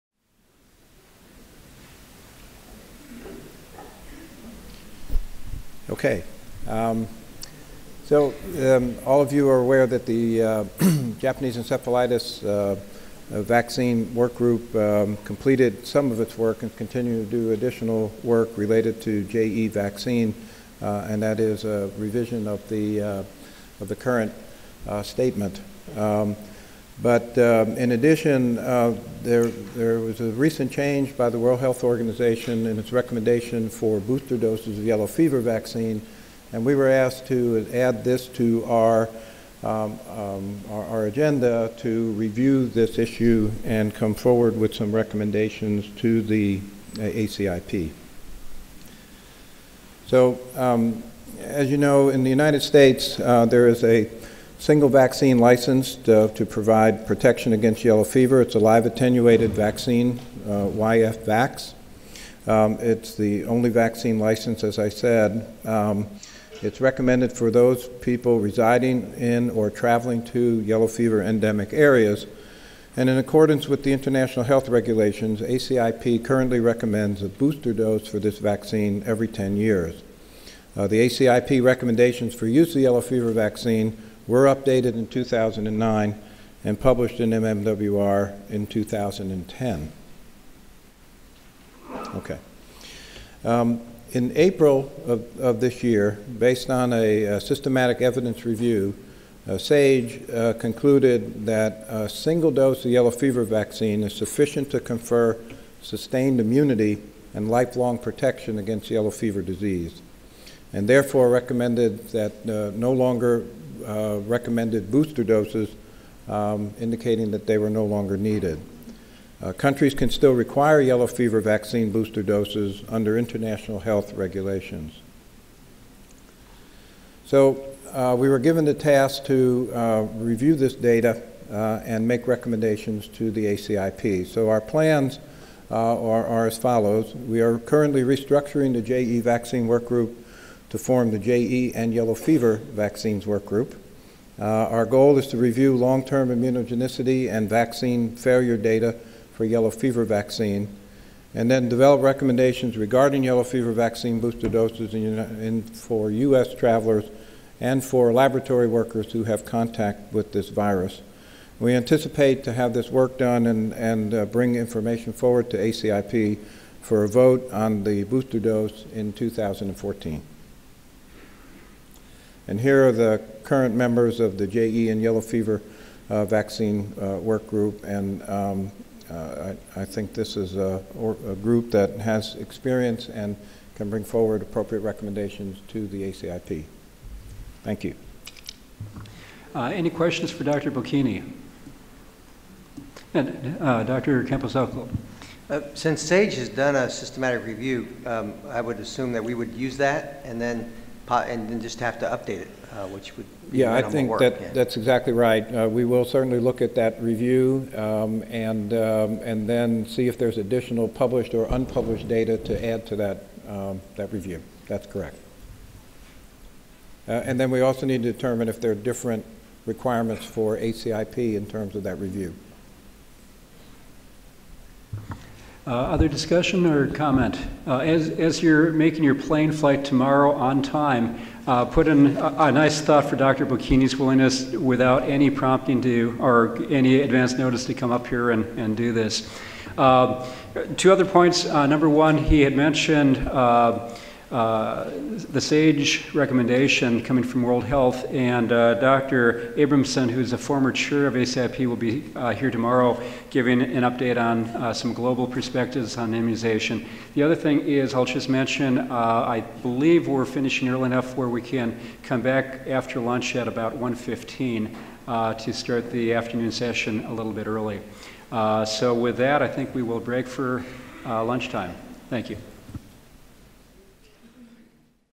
October 2013 ACIP Meeting -- Yellow Fever Vaccine
Plans to evaluate recommendations for booster doses of yellow fever vaccine by Dr. J Bocchini.